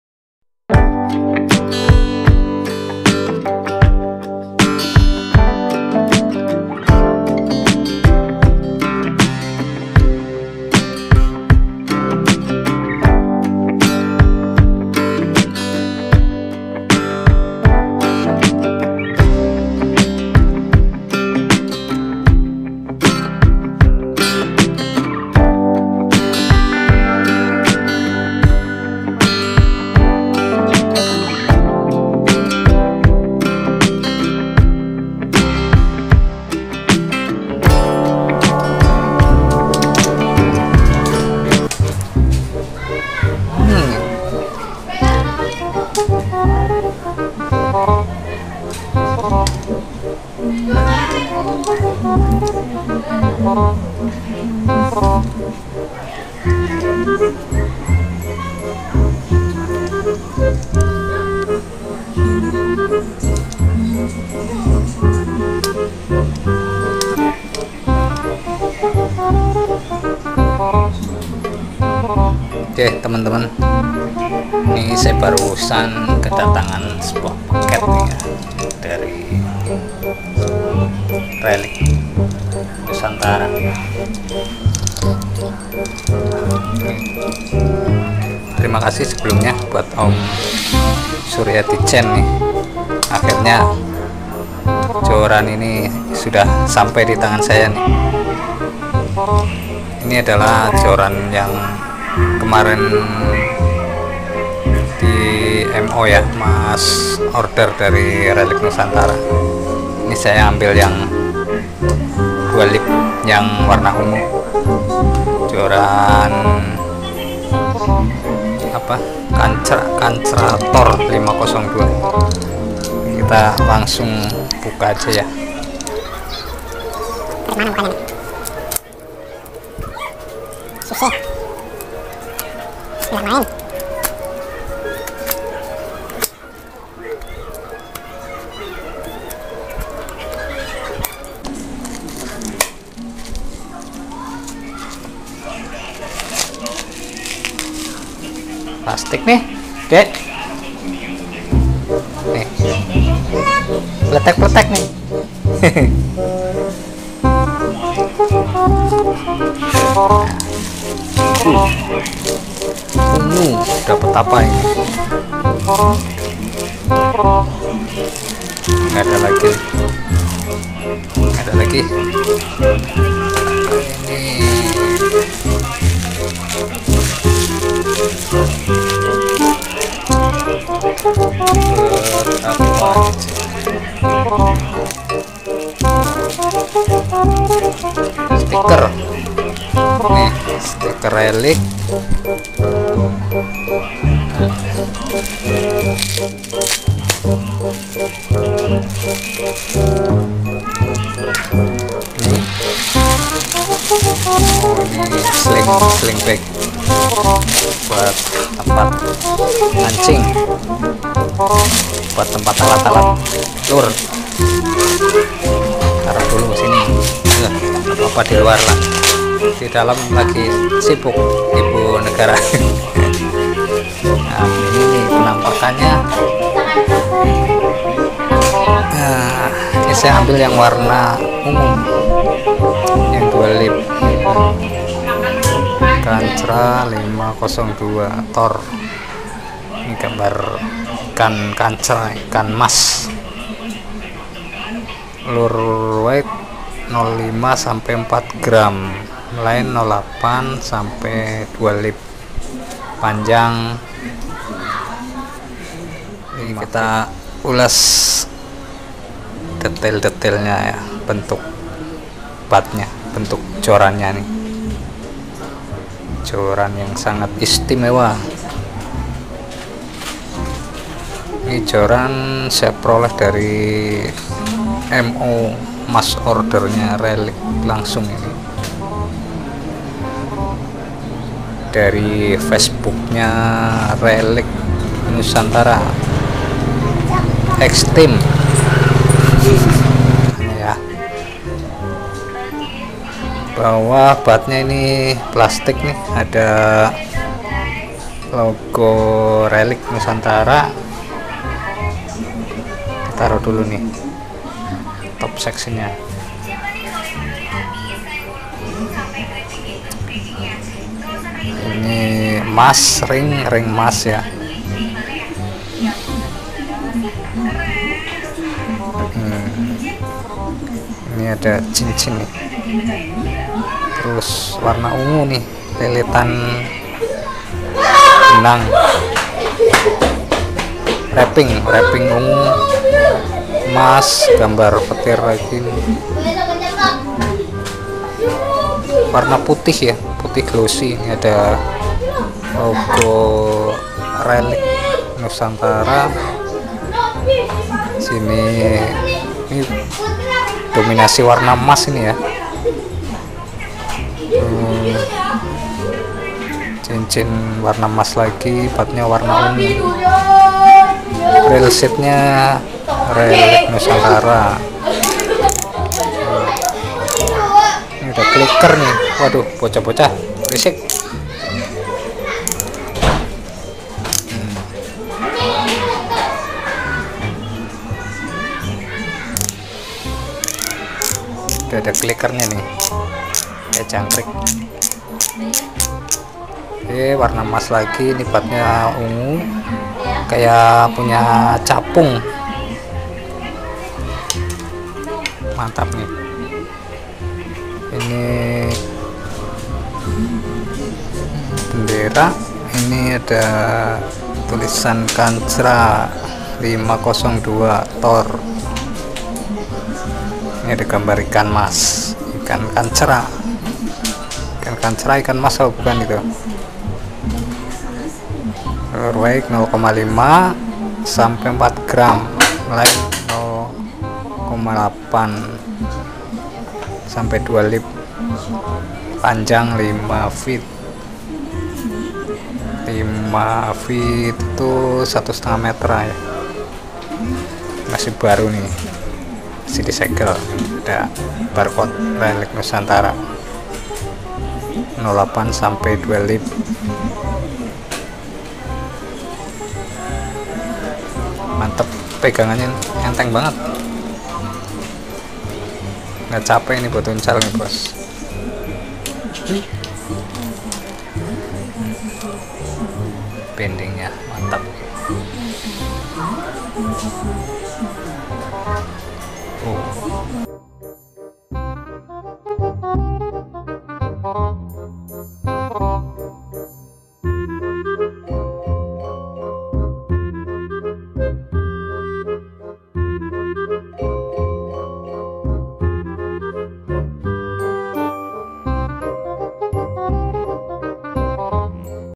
kancra tor 502 || si imut dari relix nusantara
joran kancra tor 502 0,8-2lb

unboxing joran istimewa dari relix Nusantara yg di adakan mass order pada bulan Maret, dengan harga 340rb

tag.
relix Nusantara, kancra tor, ultralight, joran relix, joran virall, joran istimewa,kancra tor,